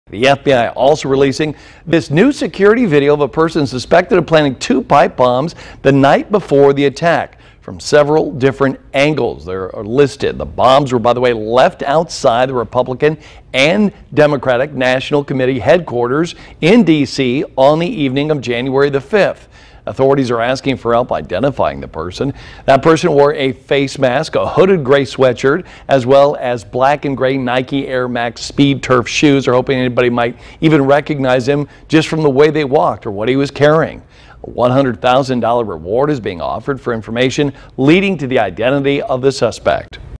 FBI releases new surveillance video
FBI releases surveillance video of a person believed to have planted pipe bombs the night before the Capitol attack.